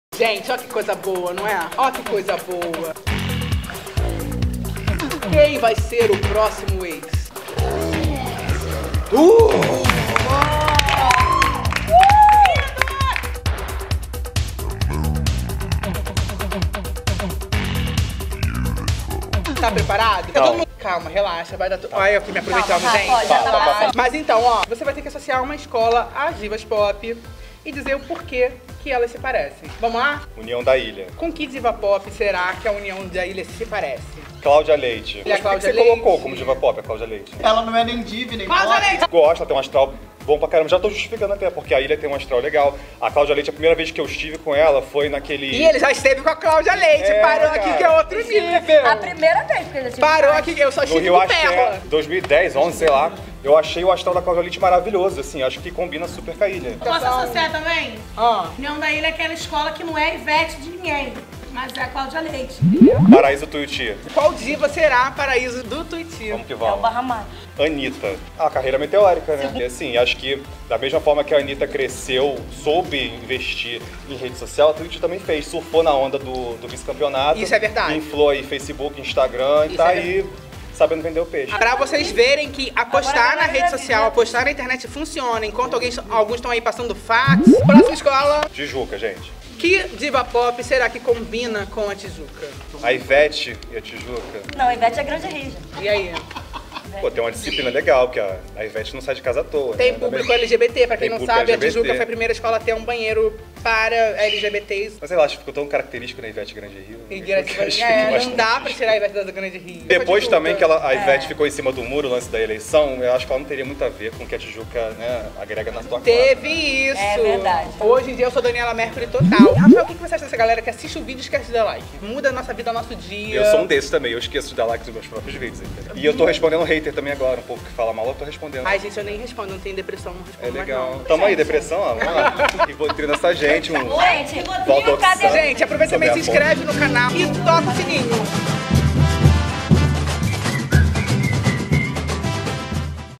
🌞 BANHO DE SOL 🌞 || Biscoitando no Carnaval (com Rafael Menezes)
No último Banho de Sol da temporada, o Pablo chamou o Rafa pra falarem de Divas Pop e carnaval